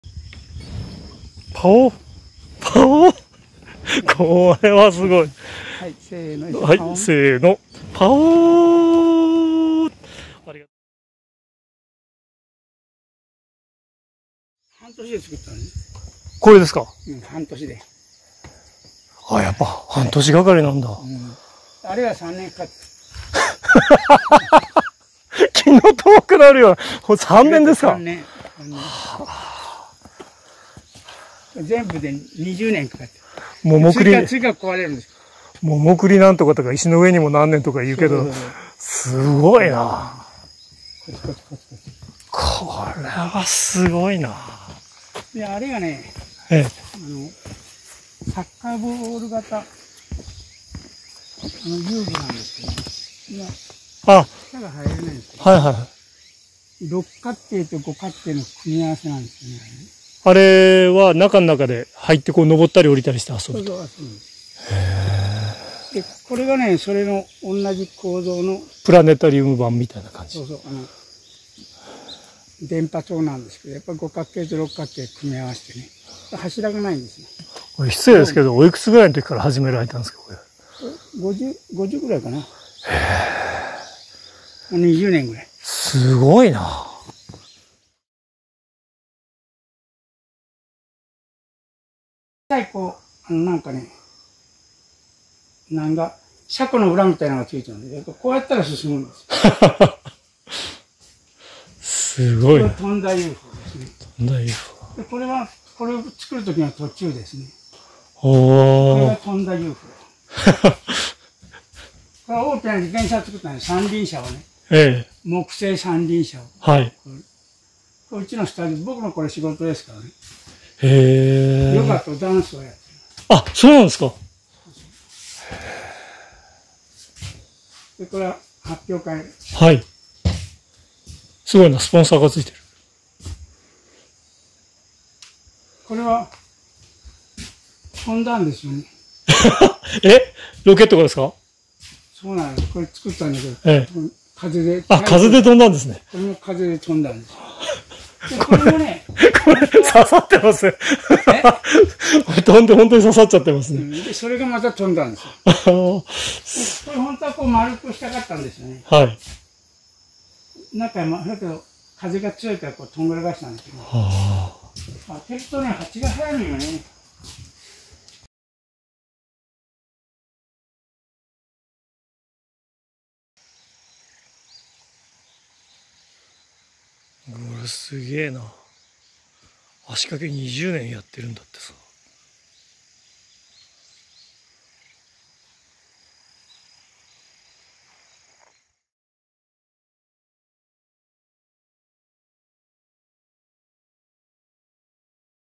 宇宙の駅〜山口県山中のUFO基地
周南市の山中でみつけた不思議空間